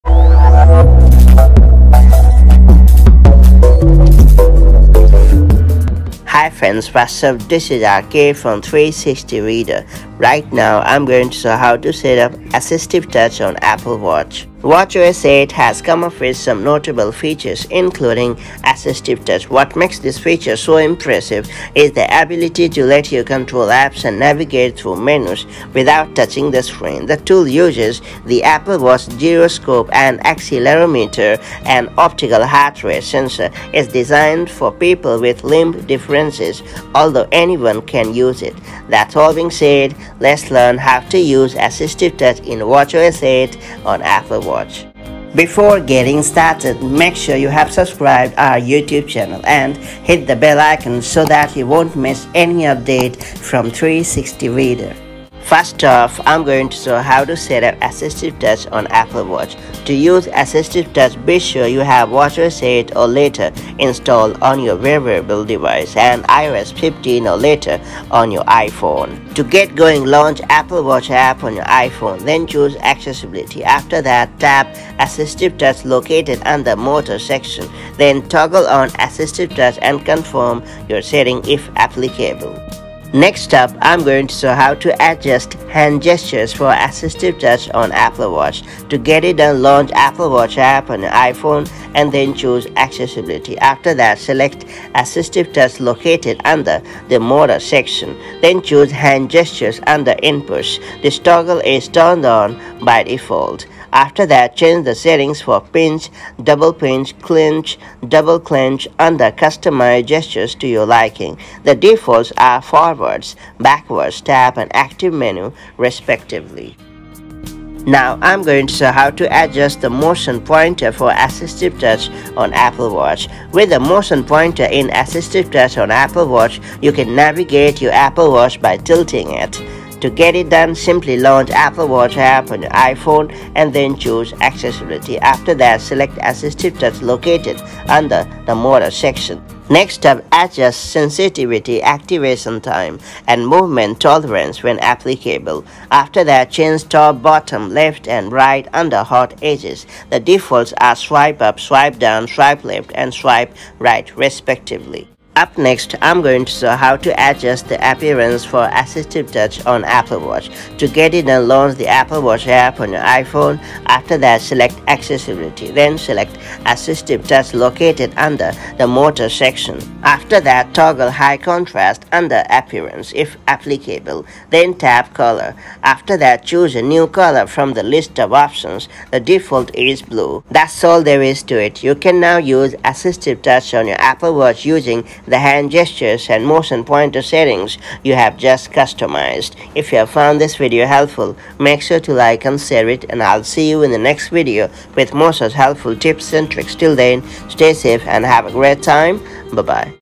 How to Set Up AssistiveTouch in watchOS 8 on Apple Watch
Here is how you can set up and customize AssistiveTouch in watchOS 8 on Apple Watch. Let’s learn how this all-new watchOS feature works!